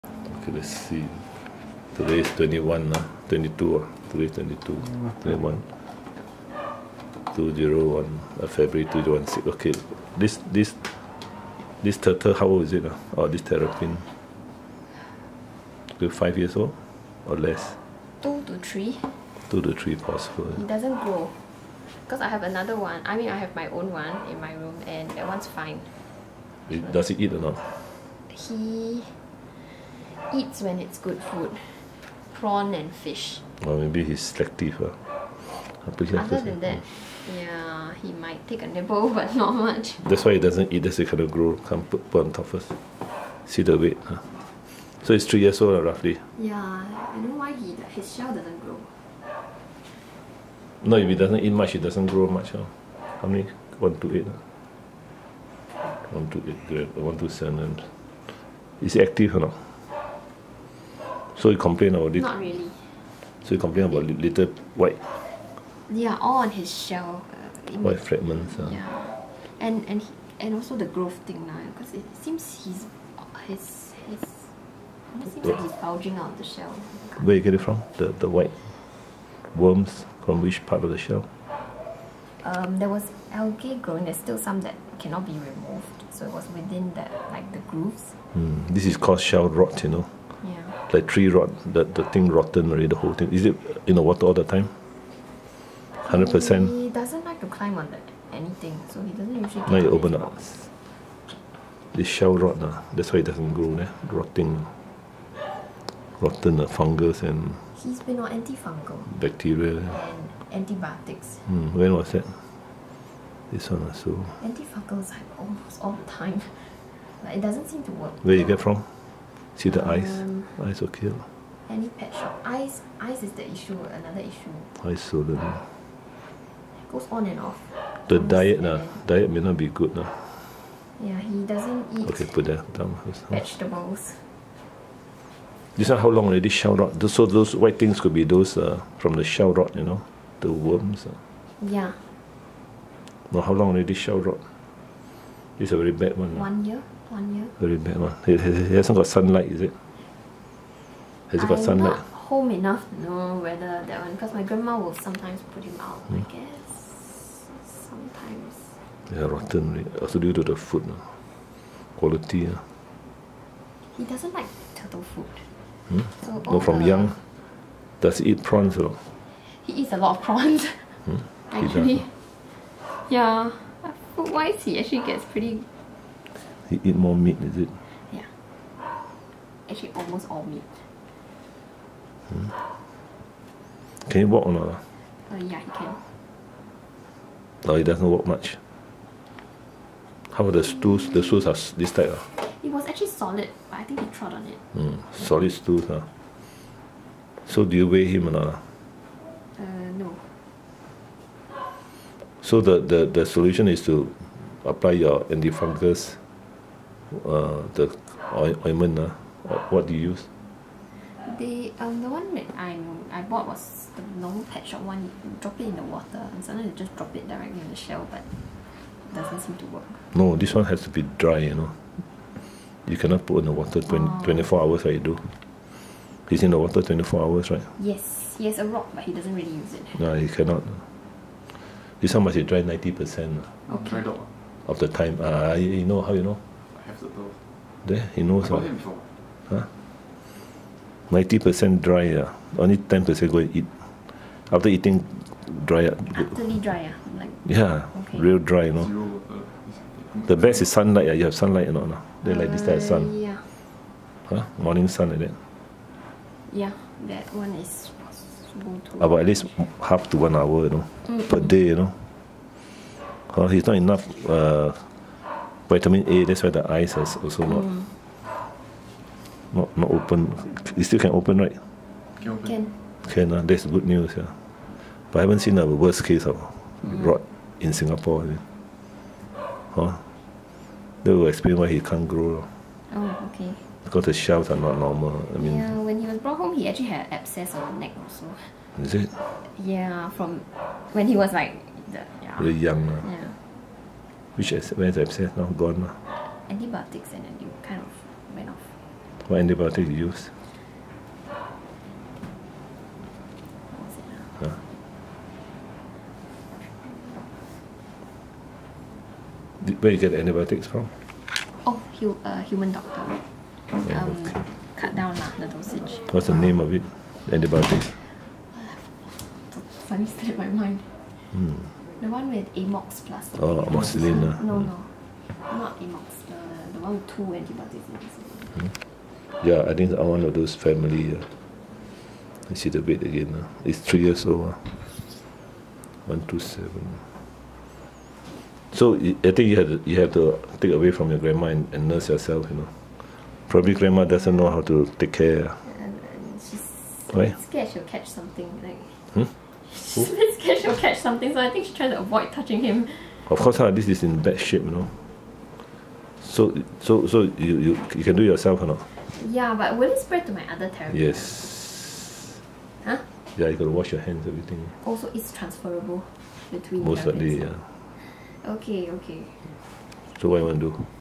Grandma's 3-year-old terrapin has shell rot Pt 1
Grandma has kept this terrapin inside a tank with water all the time. The shell has not been dry and so shell rot starts.

The grand-daughter brought in this terrapin for consultation. Dry docking 90^ of the time and anti-fungal and antiseptic wash daily must be done. Antibiotics given.